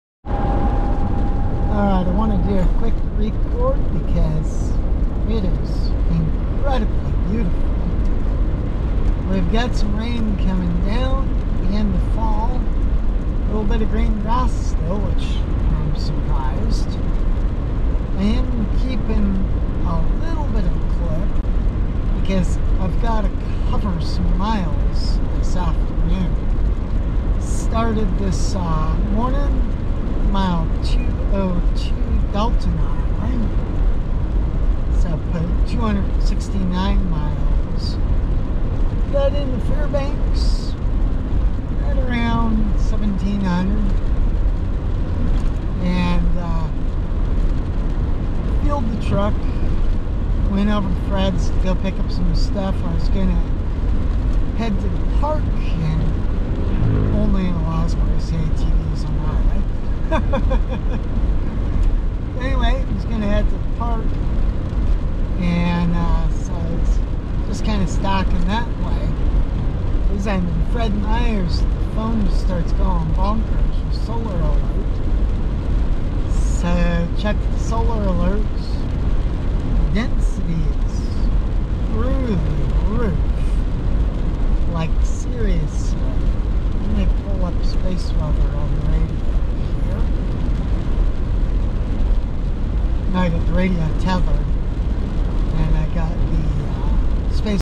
Dashcam in fall, Alaska
A quick video showing our fall colours here in Alaska